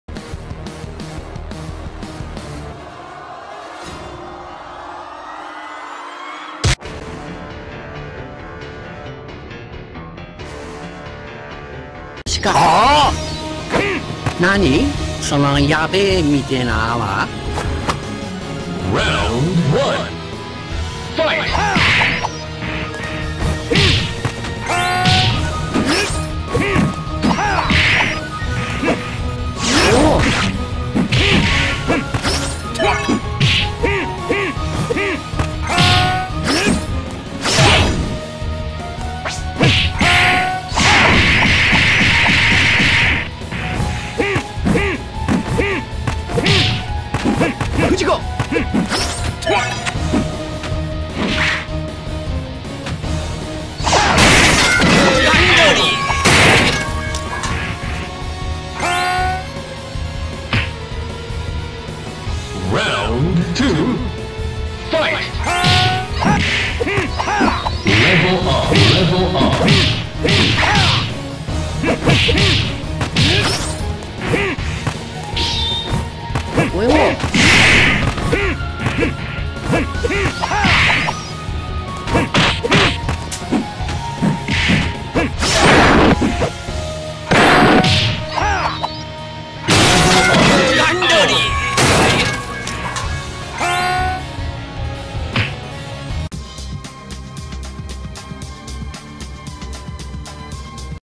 Flame Mane RMB #286 - Lupin the 3rd Vs Bowow
Well, at least he had a little bit of a fighting chance.  He needs better A.I.
Requested by maru chan aka jasmine the cute girl 2020, once again

Characters:
Lupin the 3rd by Arsène Lupin,Kain the supreme,Brucewayne74
Bowow by MrPr1993